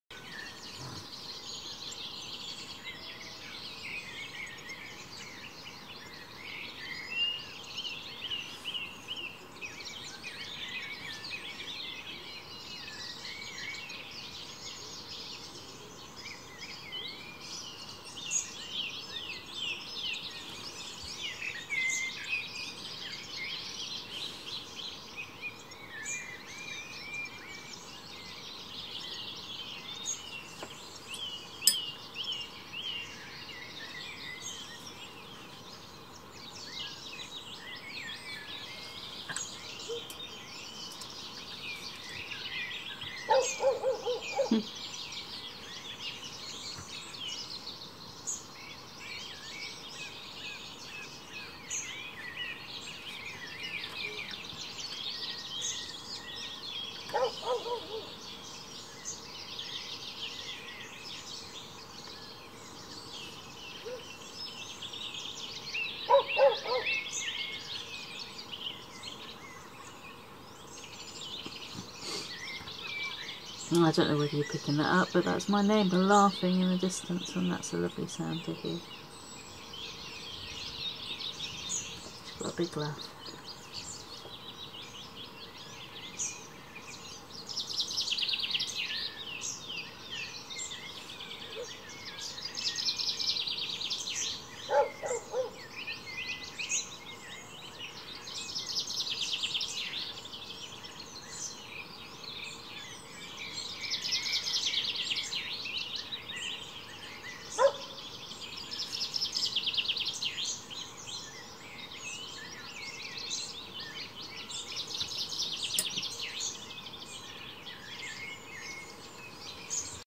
Extract from Folk by the Oak's Unwinding Stream
Watch Jackie Morris bring a painting to life, accompanied by an oak woodland dawn chorus recorded by sound artist Chris Watson - heaven!
Here is a clip from The Unwinding Stream which shows the moment that The Willow Lady’s gown is gently adorned in blues. This otherworldly painting is inspired by Jackie's beautiful new book The Unwinding.